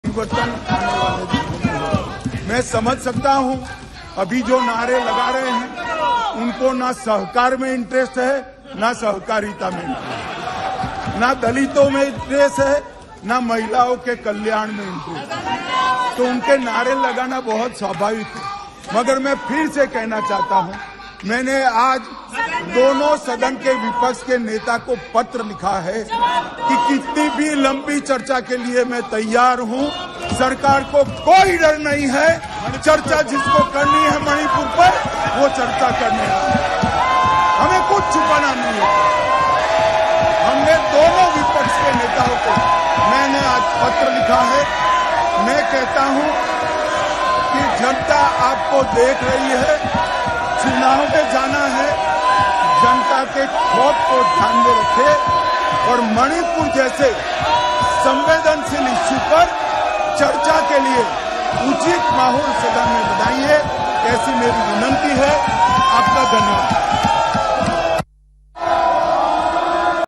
Home Minister Amit Shah Powerful Words About Manipur Incident in Lok Sabha | Parliament Live | BJP
Home Minister Amit Shah Powerful Words About Manipur Incident in Lok Sabha | Parliament Live | BJP | YOYO TV Kannada Lok Sabha